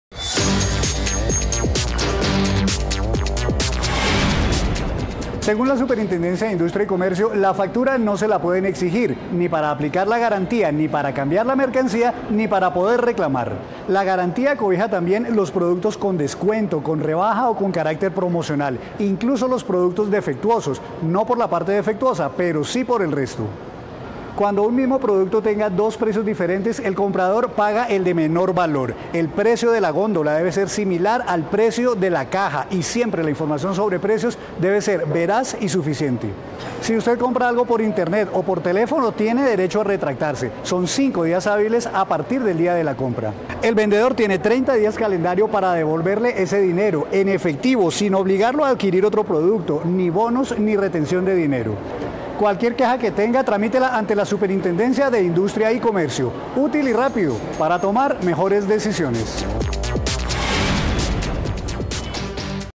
¿Sabe cuáles son sus derechos como comprador?
Emitido el 15 de Marzo de 2017. Tenga en cuenta que la factura no es obligatoria para aplicar la garantía de un producto, ni para un cambio de mercancía.La garantía debe cobijar los productos con descuento, rebaja o con carácter promocional.Cuando un mismo artículo tiene dos precios diferentes, el comprador paga el de menor valor, el precio en la góndola debe coincidir con el de la caja.Si compra por internet o por teléfono tiene derecho a retractarse en cinco días hábiles, a partir del día de la compra.El vendedor tiene 30 días calendario para devolverle ese dinero en efectivo, sin obligarlo a adquirir otro producto, ni bonos ni hacerle retención de dinero.